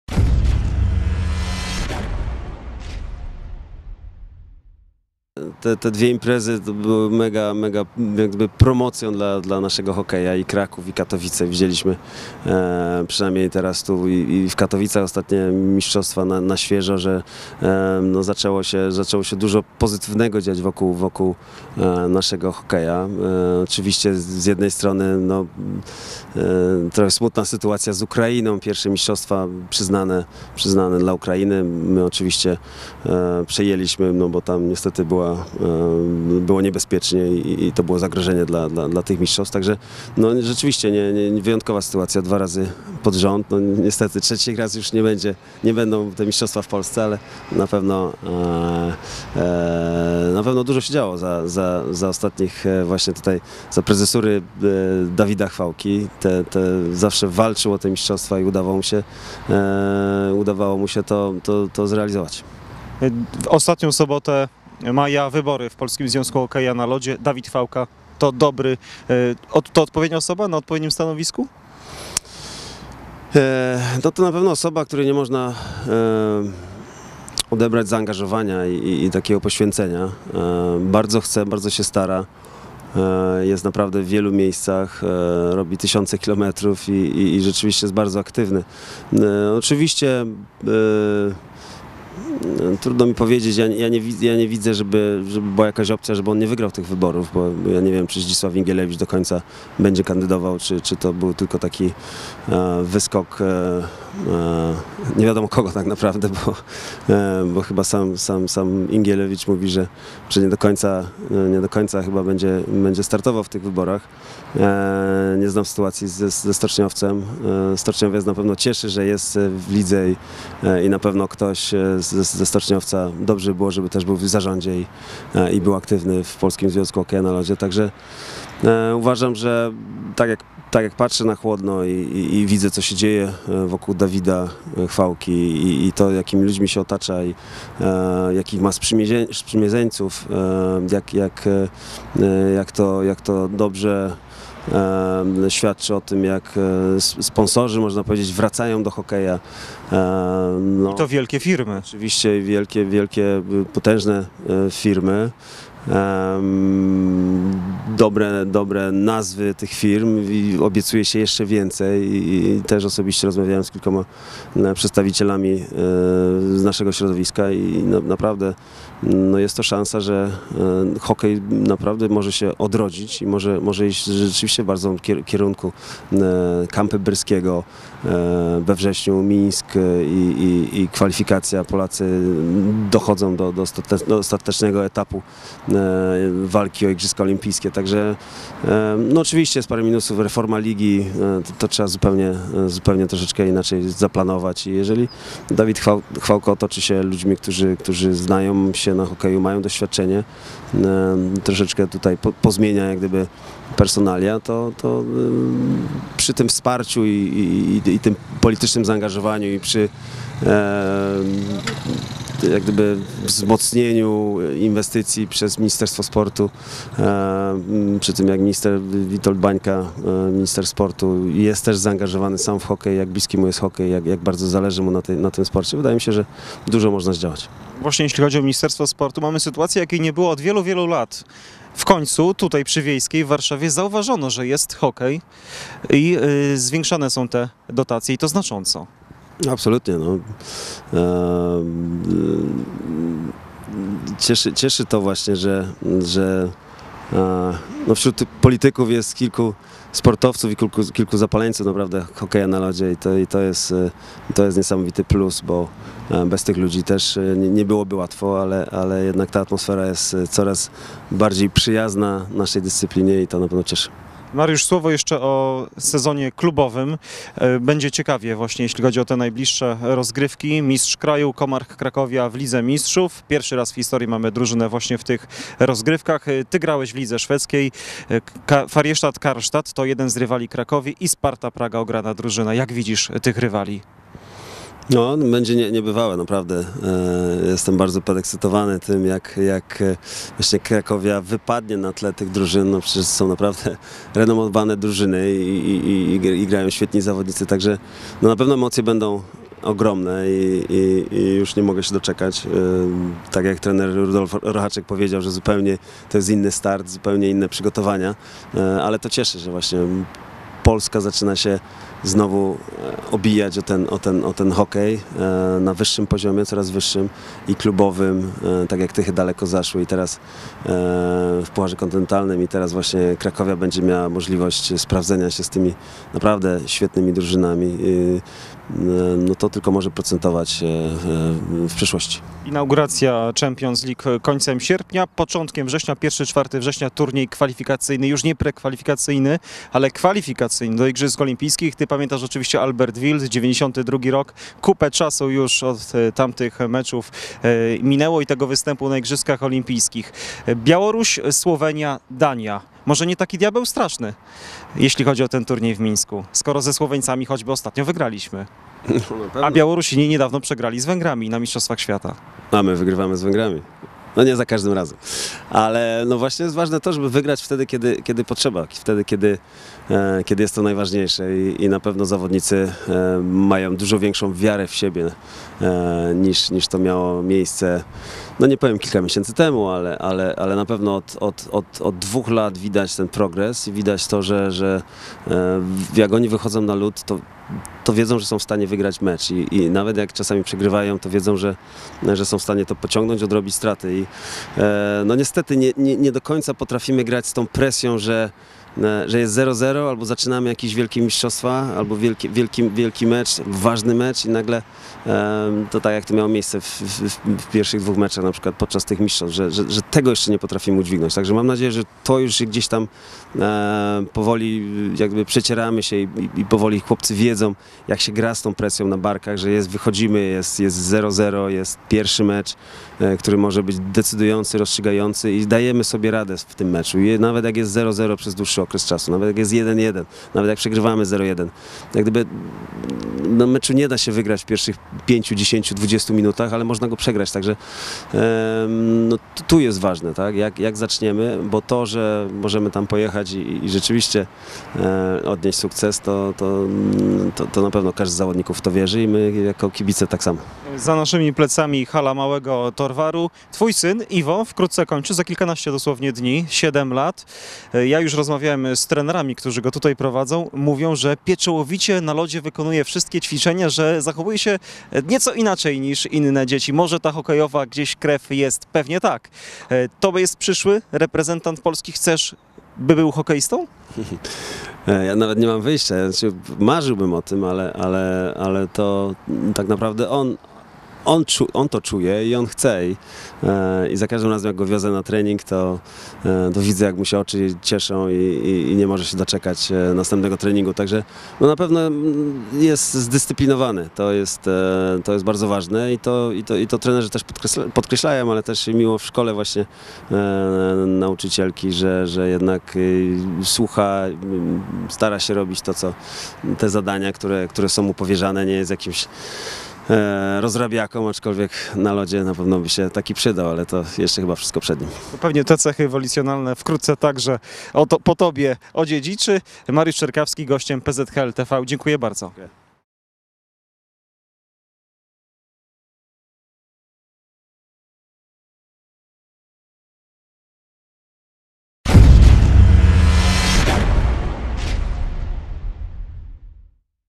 [ Mariusz Czerkawski chwali ostatnie lata w polskim hokeju
Mariusz Czerkawski, były znakomity hokeista a obecnie ekspert i komentator telewizyjny, w rozmowie z serwisem chwali ostatnie dwa lata Polskiego Związku Hokeja na Lodzie pod rządami prezesa Dawida Chwałki.